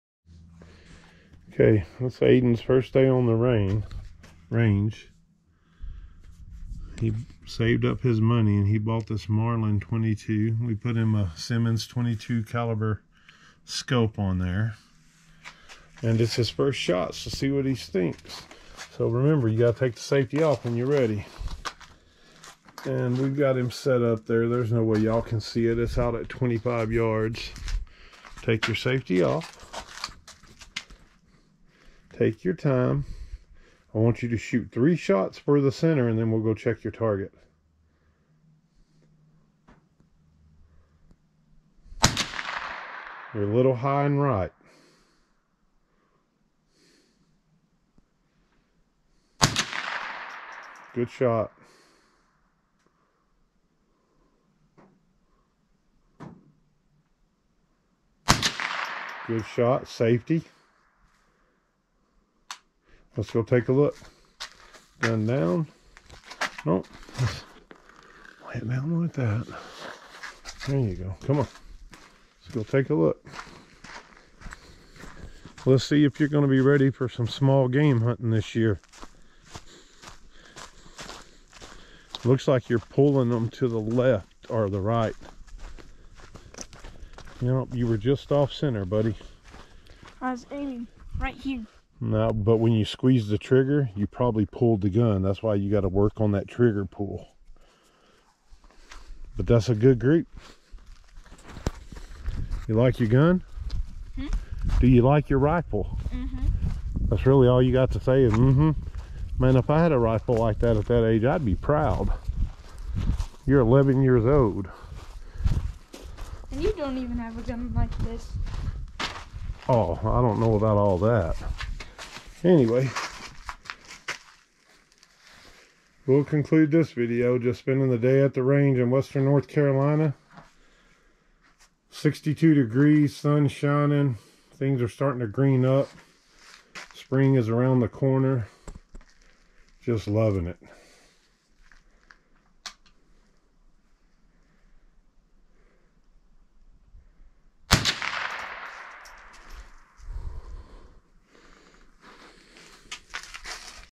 Marlin .22
My son shooting first semi-auto Rifle he saved half the money I gave him the other half for his Birthday